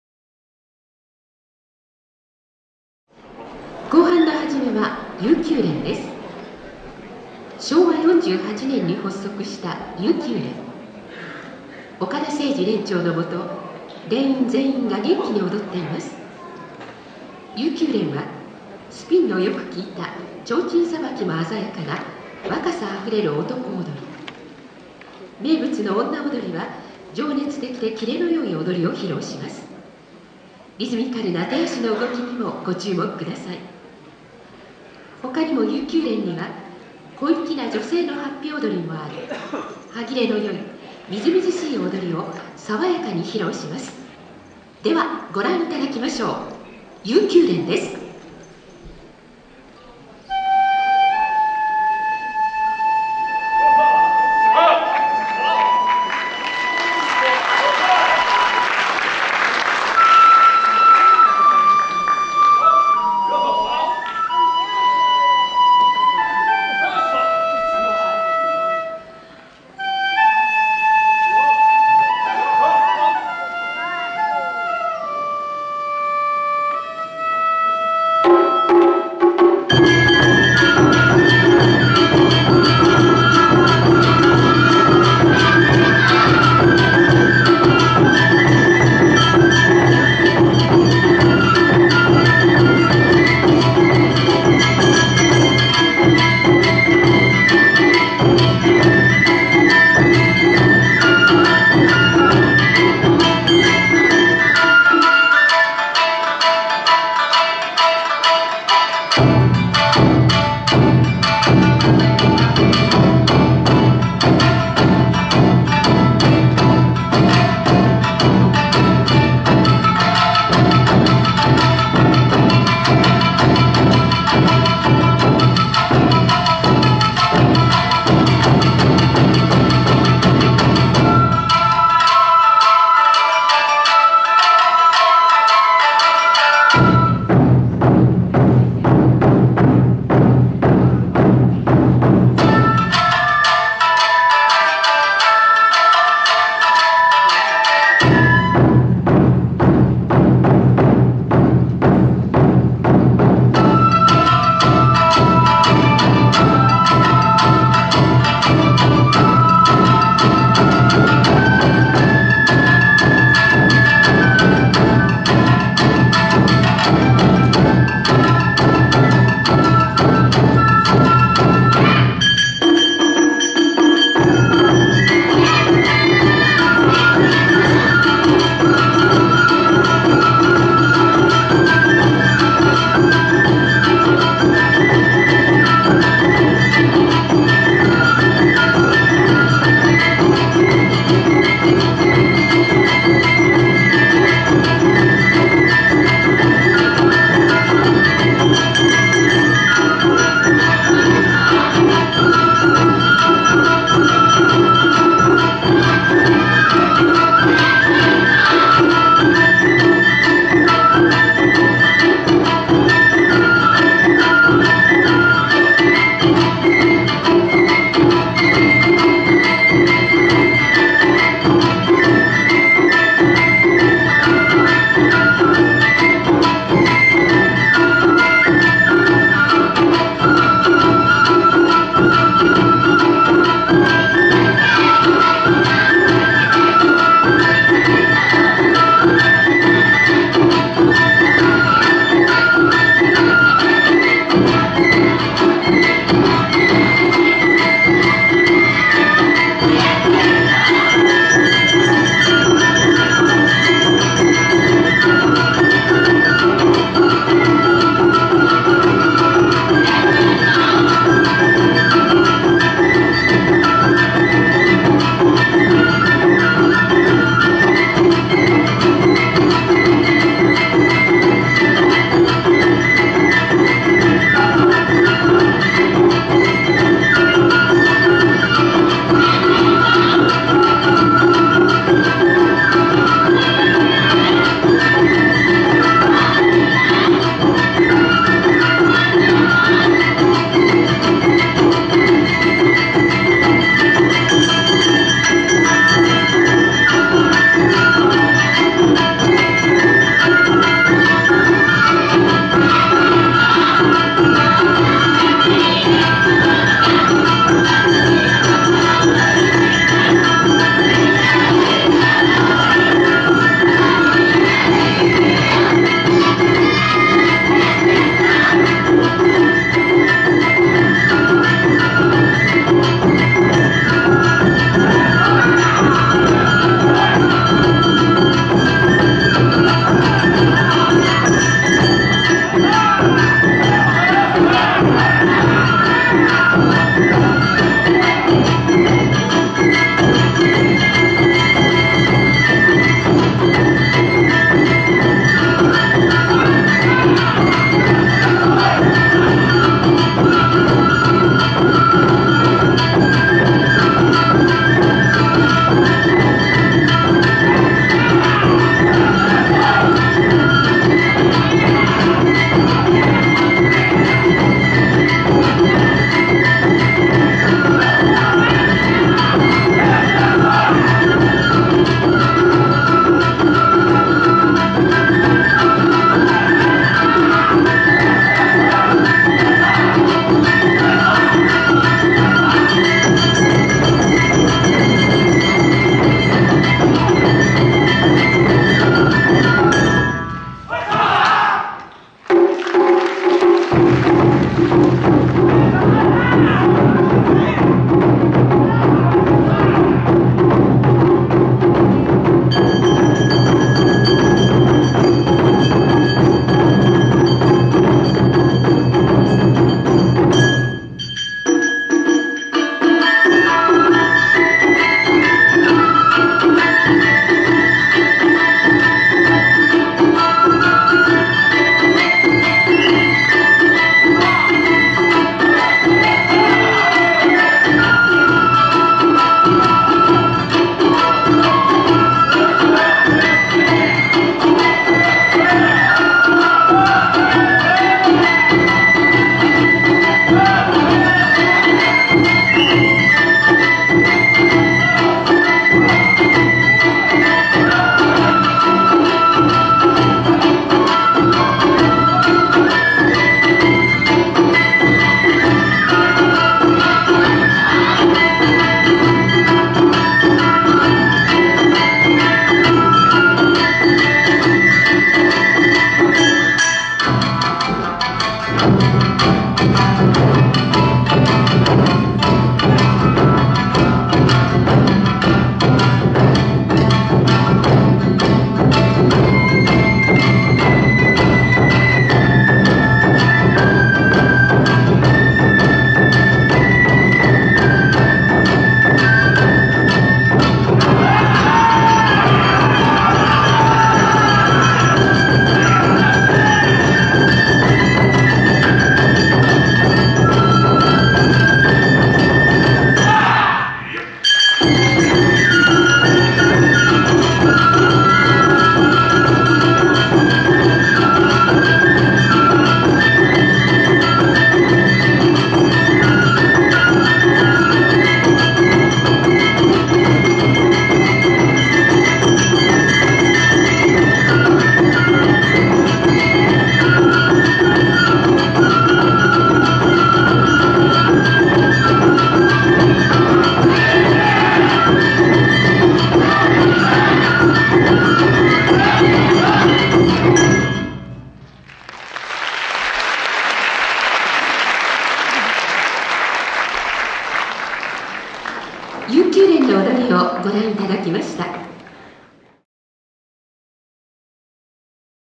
「悠久連」 はなはるフェスタ2012 阿波おどり春舞台
～とくしま春らんまんの祭典　はな・はる・フェスタ２０１２「阿波おどり春舞台」～
春に咲く、二日限りの阿波おどり。
夏の阿波おどりに次ぐビッグイベント！
有名連33連が春ならではの華やかなステージを繰り広げました。

撮影日：2012年4月21日 第１部
撮影場所：あわぎんホール
出演：悠久連